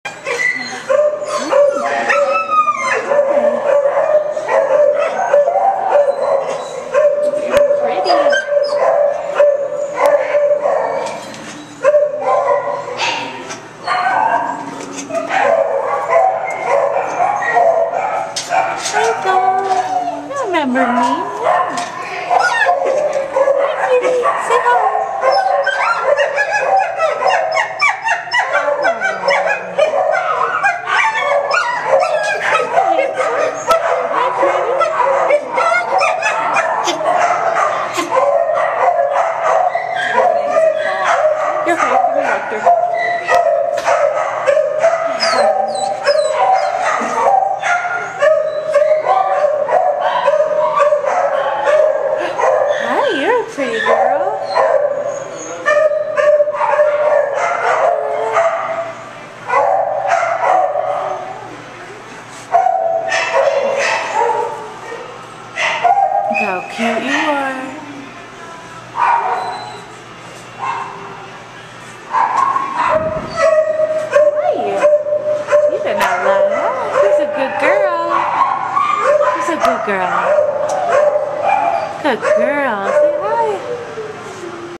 Animal Welfare League in Chicago, April 17, 2011 - female dogs who need a home
These beautiful dogs are at Animal Welfare League on the South Side of Chicago and are just waiting for someone to rescue them. This is ground zero for stray/lost/given up dogs in Chicago. Many loving dogs here. They also are in great need of donations, whether it be cash or dog/cat food, cat litter, blankets, dog beds, a washer and dryer (and a hook up--the building is very old), bathing products, medical supplies, flea/tic meds, etc. Contact @meaghanolson via direct message on Twitter if you can donate anything. Thank you!